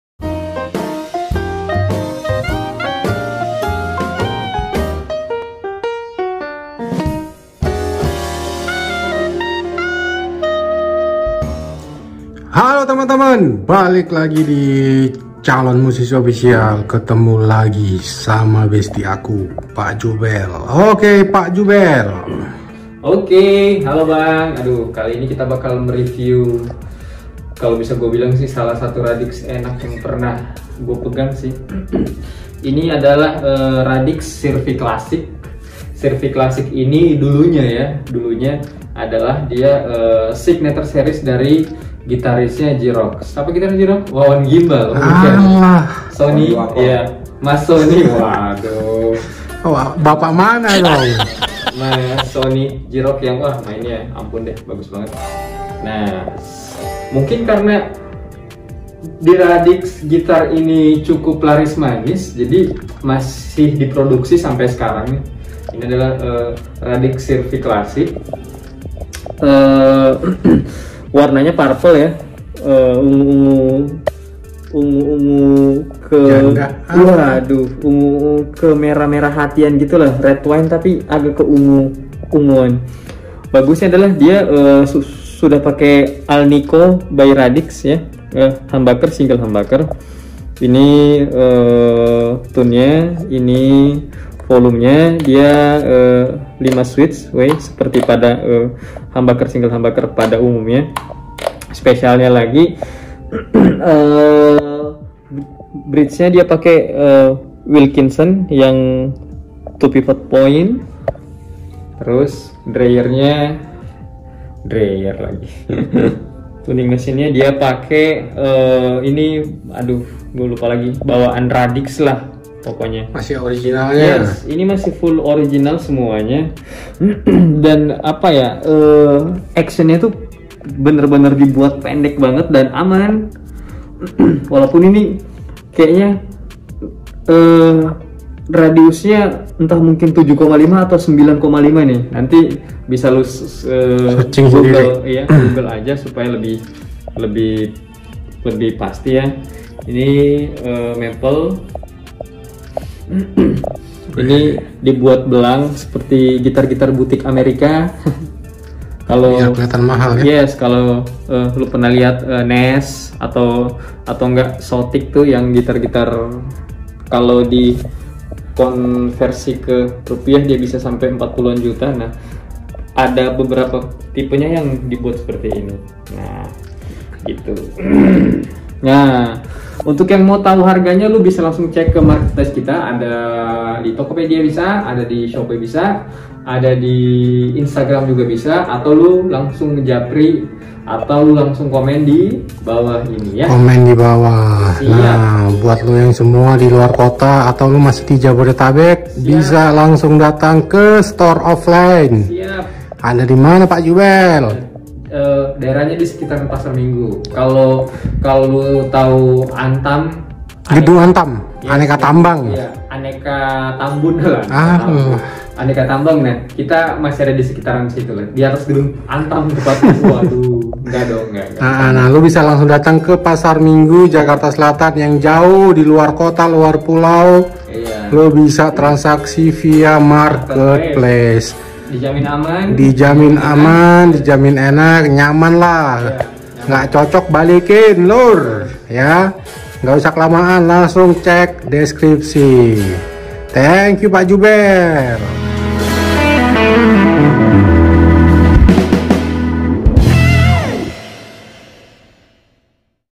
REVIEW GITAR BEKAS BUATAN ANAK BANGSA‼️| Radix Guitars Classic Sir-V Series | CALON MUSISI
Halo temen-temen..✌
Buat temen² kalau ada request harus review gitar atau tutorial gitar apa silahkan tulis dikolom komentar ya☺️

Salam,
CALON MUSISI❤